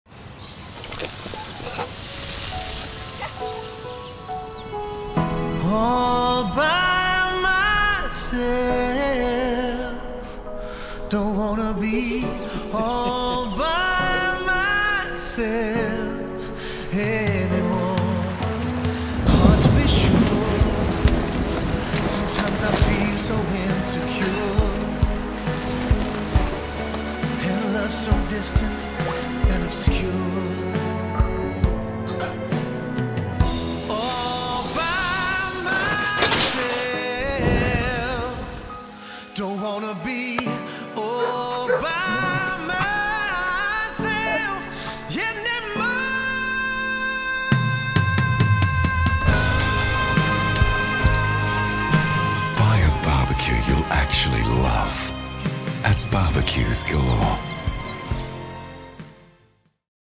'Buy a BBQ you'll actually love': BBQ Galore 60" campaign
Iconic Aussie retailer Barbeques Galore gets all the love in this new campaign from The General Store.